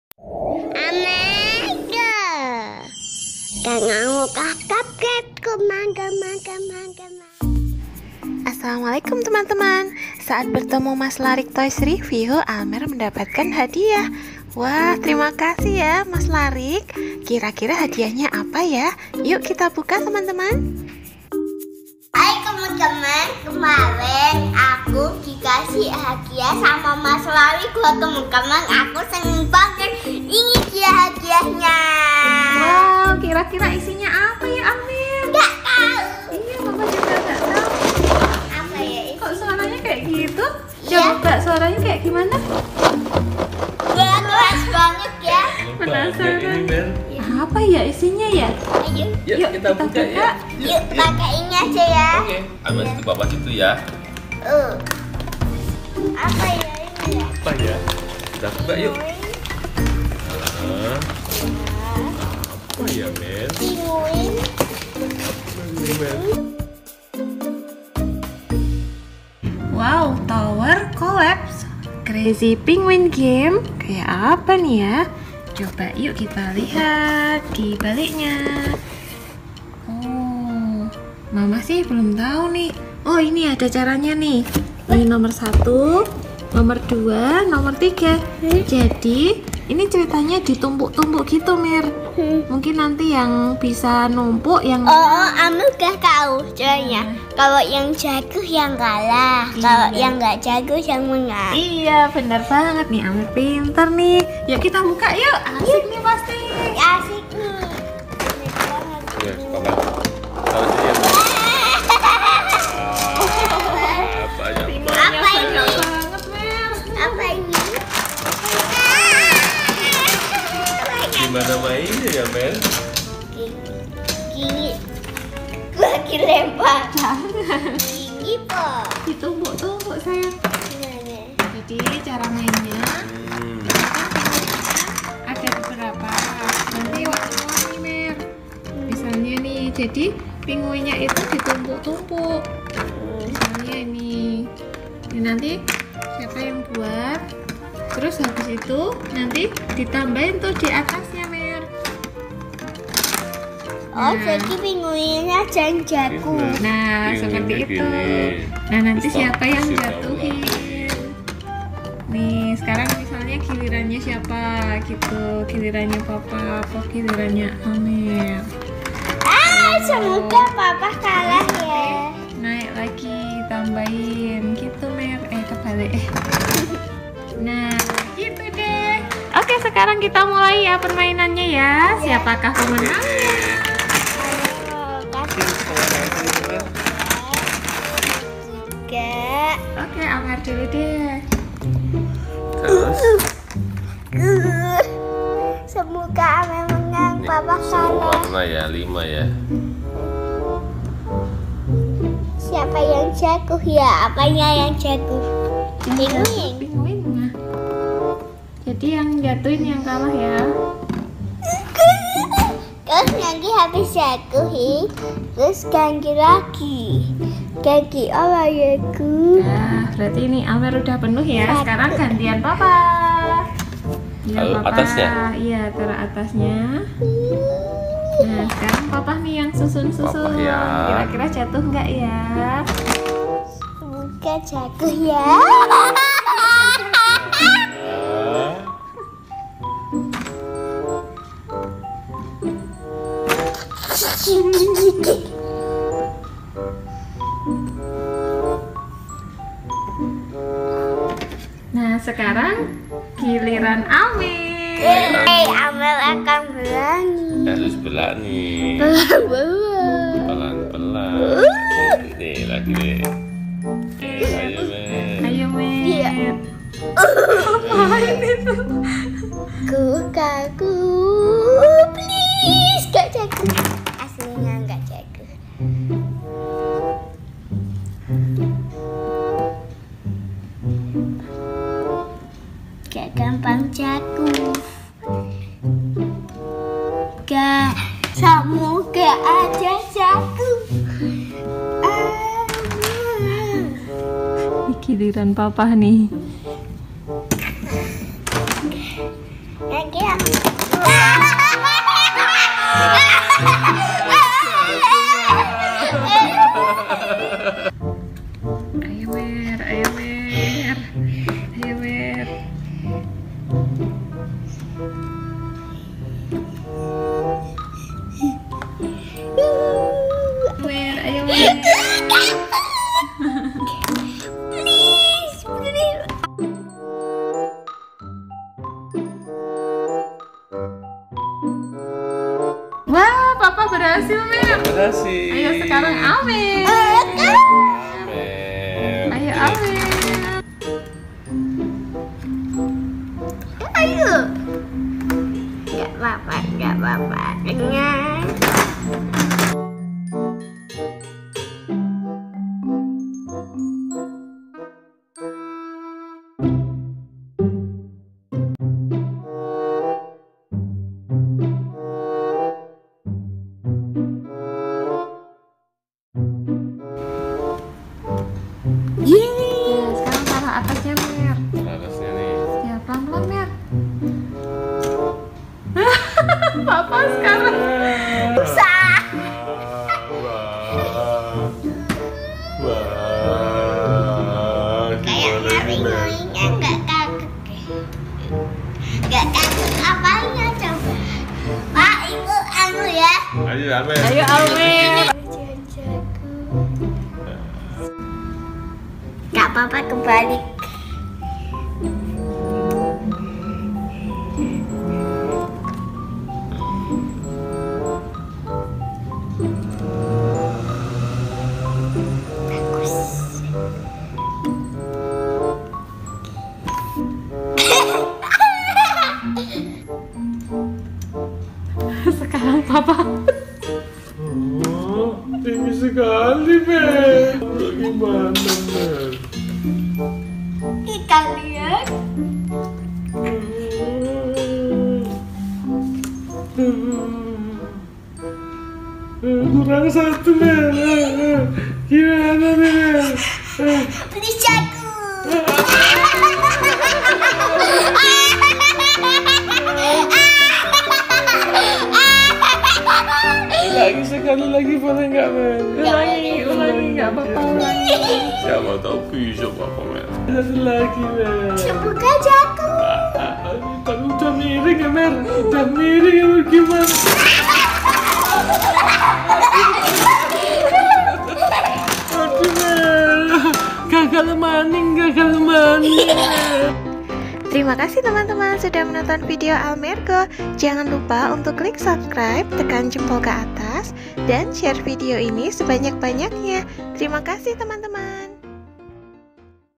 HADIAH Dari Larics Toys Review - PAPA atau ALMER yang MENANG ? Mainan Tumpuk Pinguin | ALMER GO
Saat bertemu dengan mas Larics almer dapat hadiah, terimakasih mas larics. Setelah dibuka ternyata mainan pinguin yang bisa ditumpuk tumpuk. Almer bermain bersama Papa. Kira kira siapakah pemenangnya? Almer atau Papa ya ?? lihat video serunya yuuk